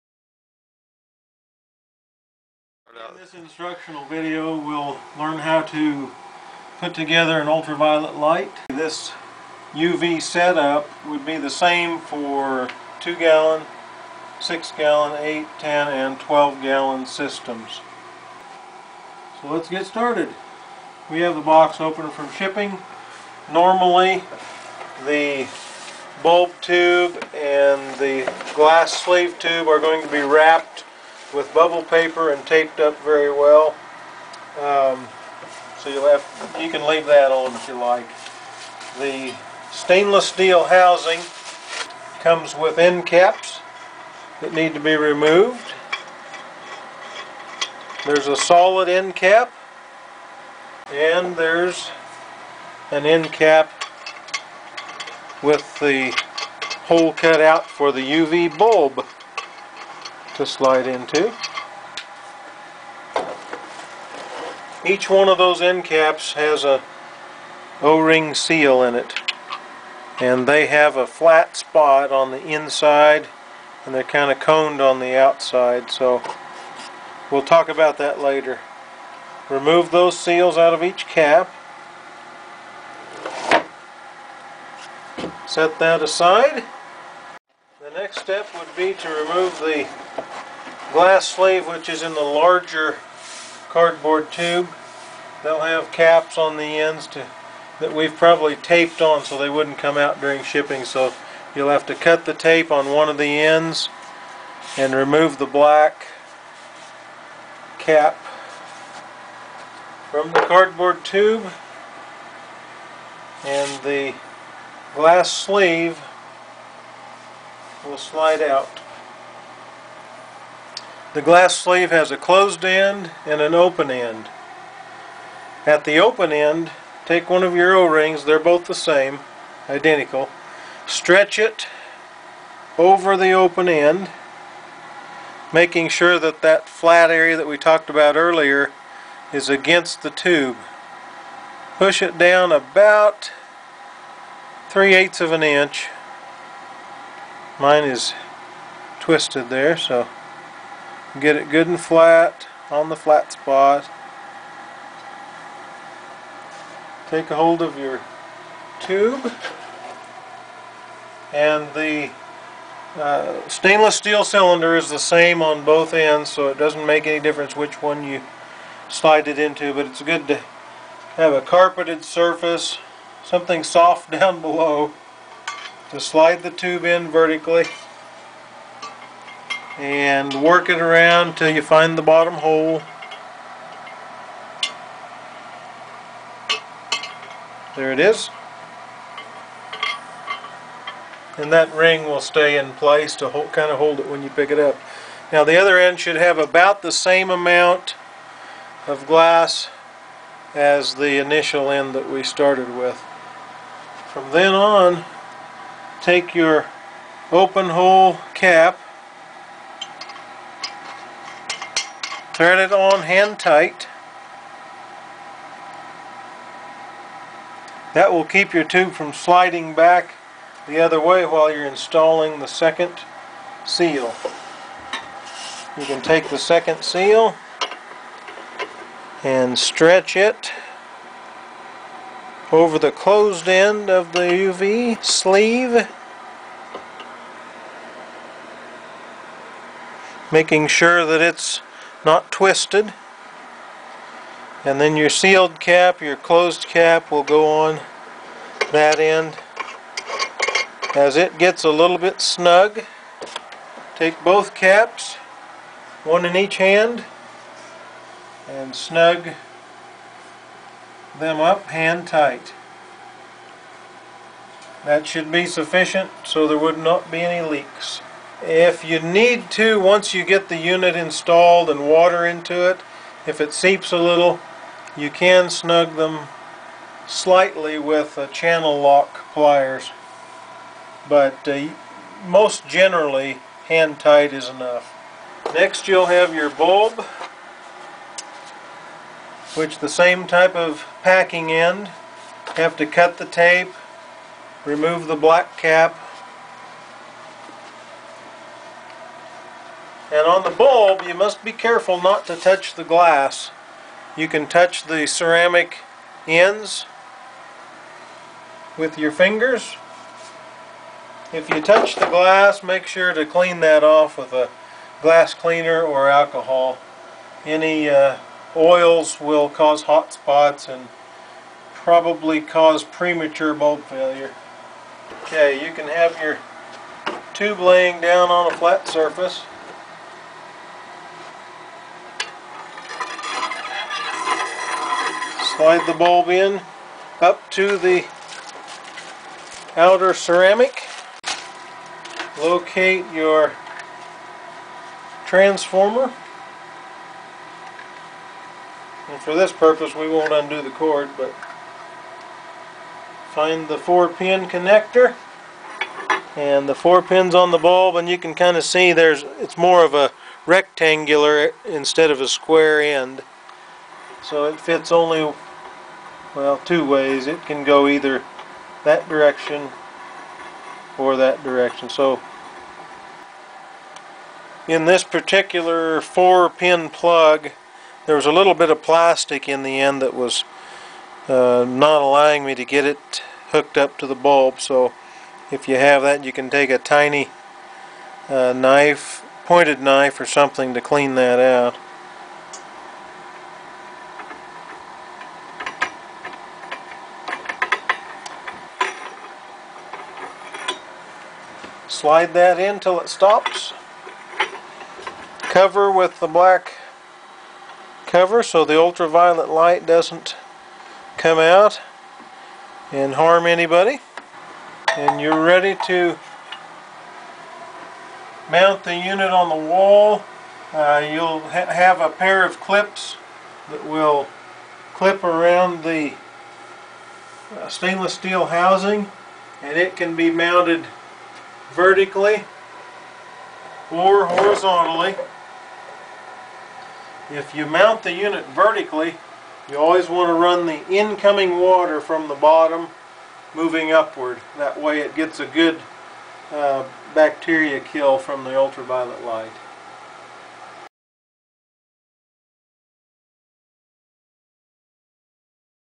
Ultra Violet Sterilizer Preparation for 2, 6, 8, 10, and 12 Gallon Per Minute Sterilizers H2O Splash
NOTICE: Both caps are now open ended, there is no closed cap. Unit is now shipped with o-rings and glass sleeve installed inside the housing.
This is how you should best prepare your sterilizer for use.  If you need more info on something or have a video suggestion that we should record tell us in the comment and we will see if we can help you more.